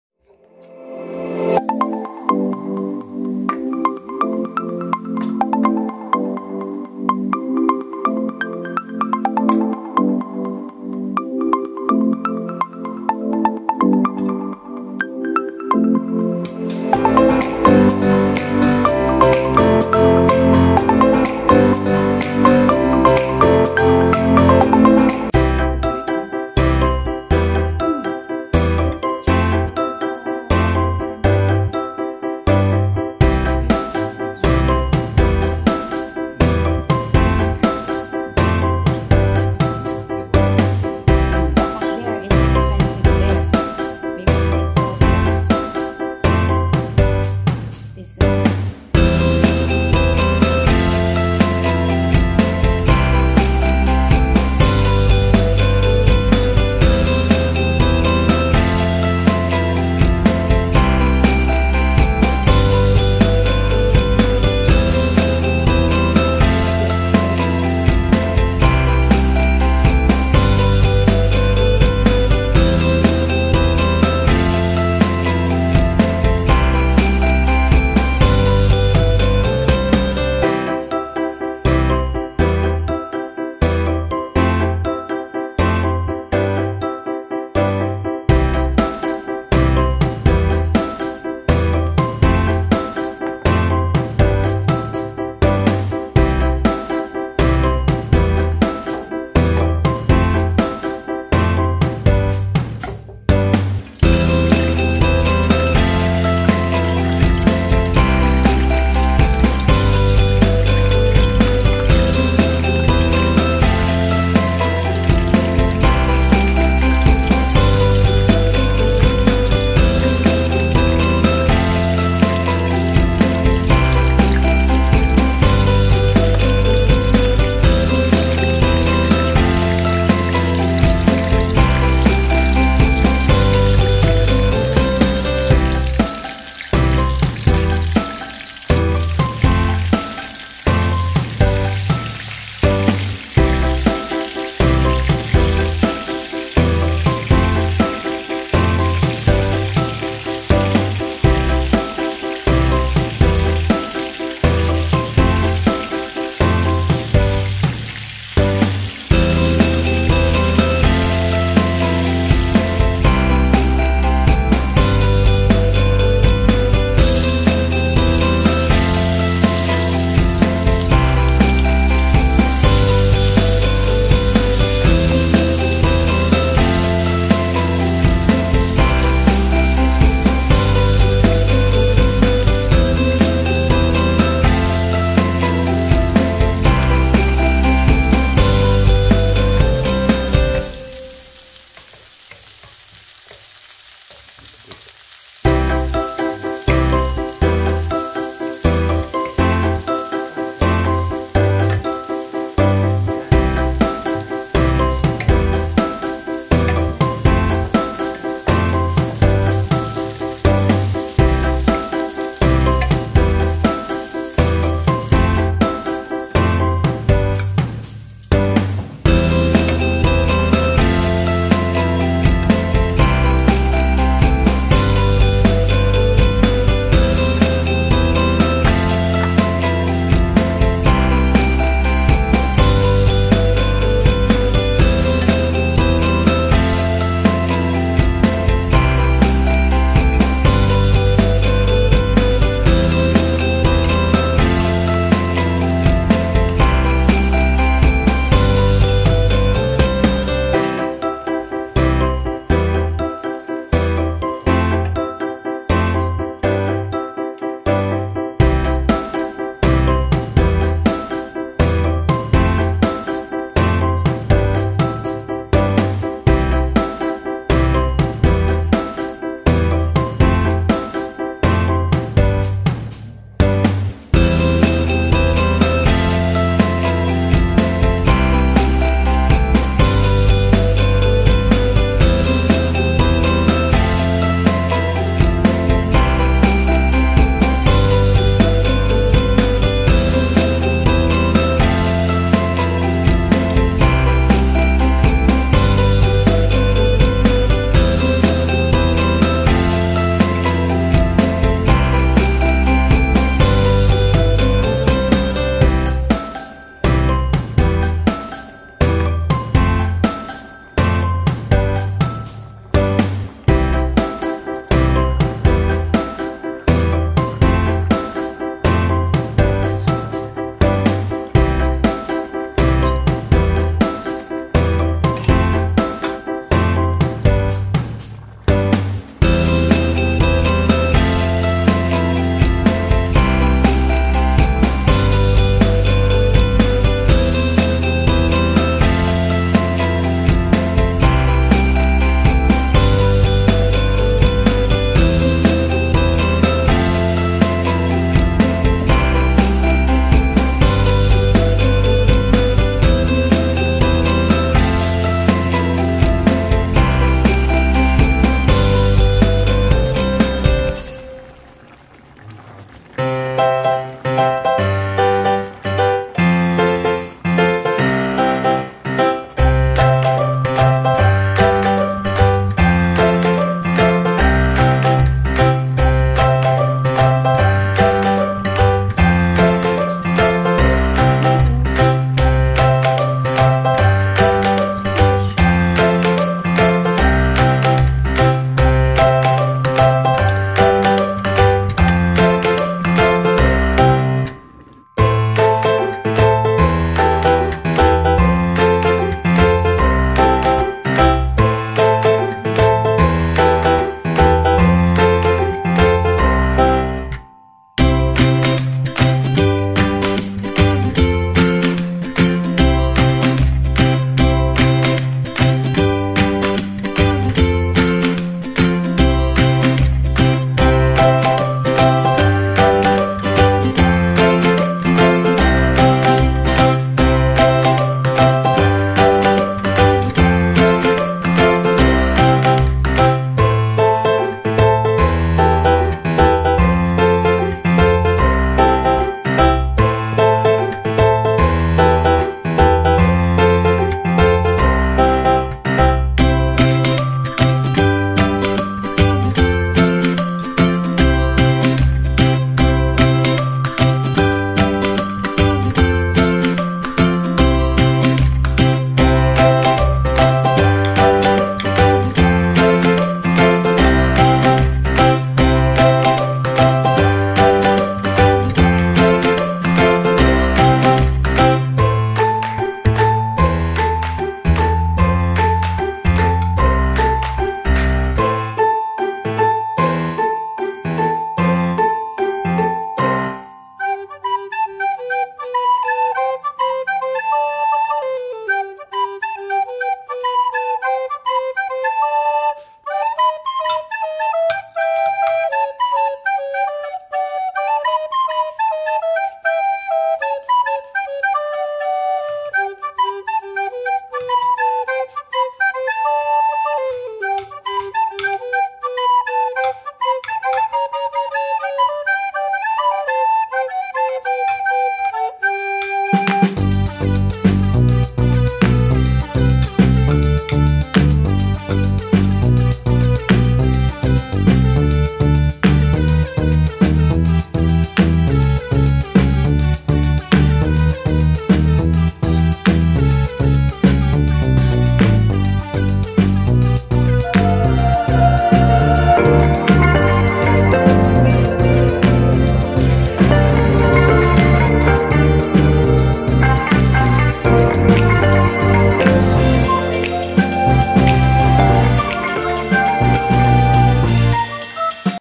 HOW TO COOK BONITO FISH IN COCONUT MILK FISH IN COCONUT MILK TULINGAN SA GATA
Ginataang Bonito or Tulingan is a delicious fish dish cooked with onion, garlic, vinegar, coconut milk, eggplant, bok choy and green Thai Chili.

Let me know if you like dishes with coconut milk too on the comment section below👇 what dish did you make ,share with us!